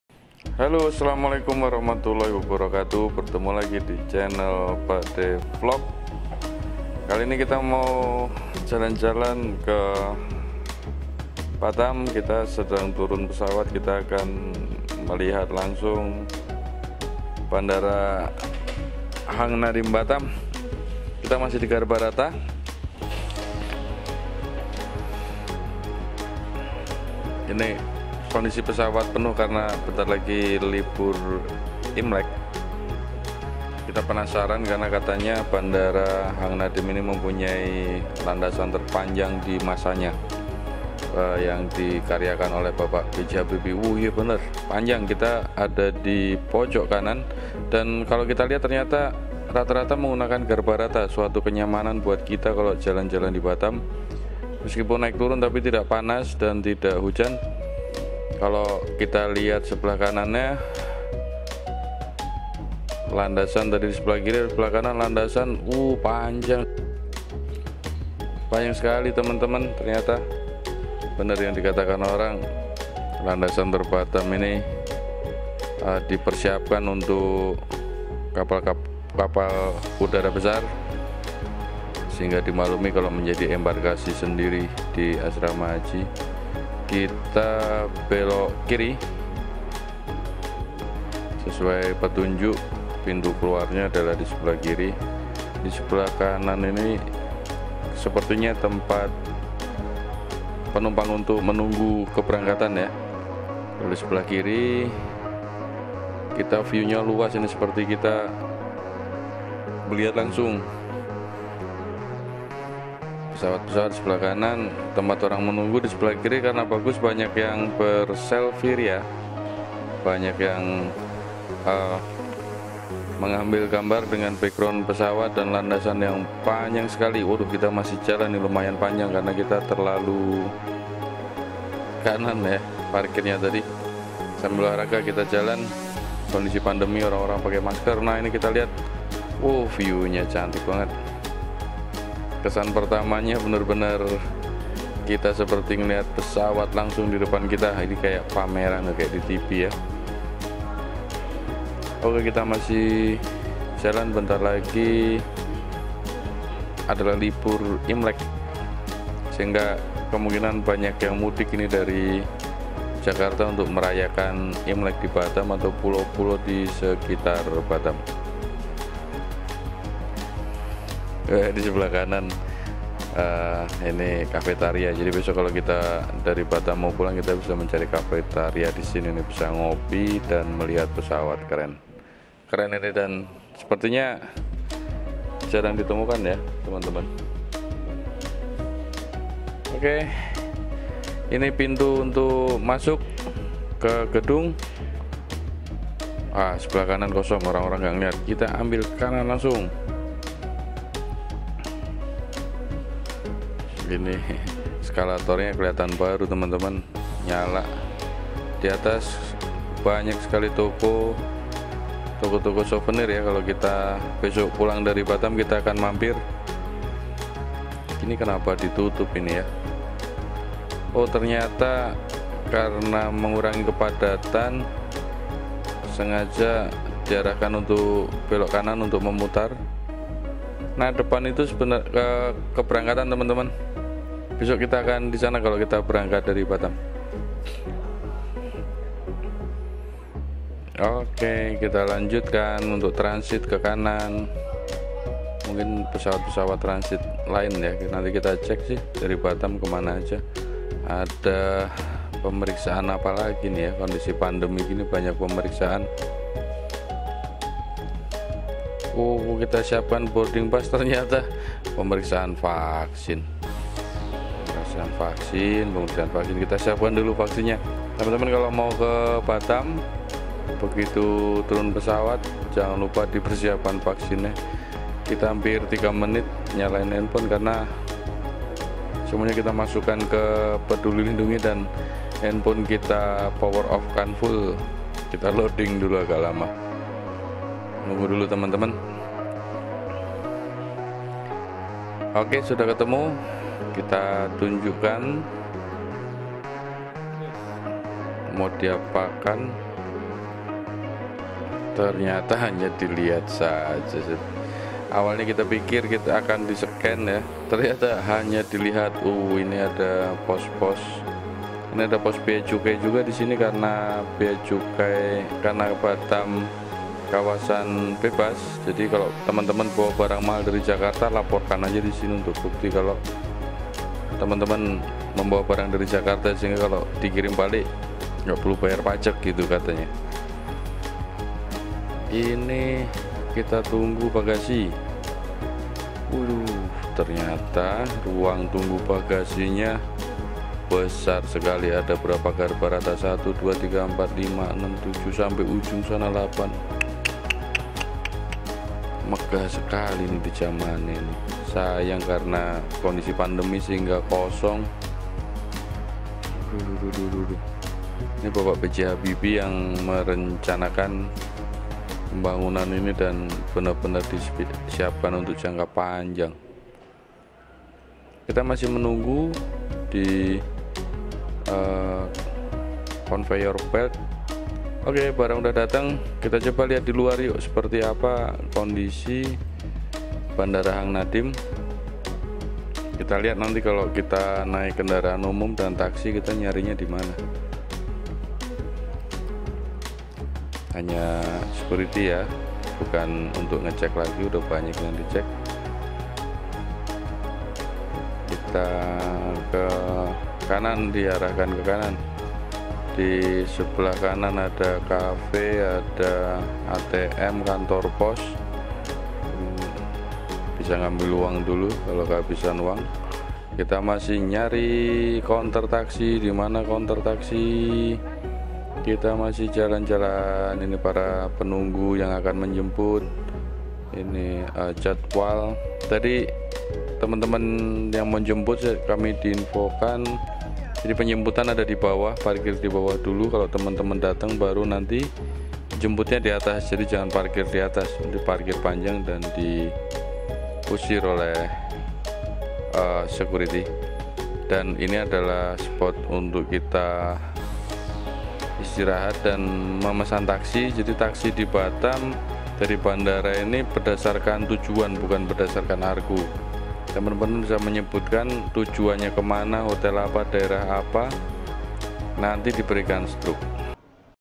FIRST IMPRESSION BANDARA HANG NADIM BATAM
Suasana bandara Hang Nadim Batam, first impression, melihat suasana kedatangan Bandara Hang Nadim dan Loket Taksi Bandara Hang Nadim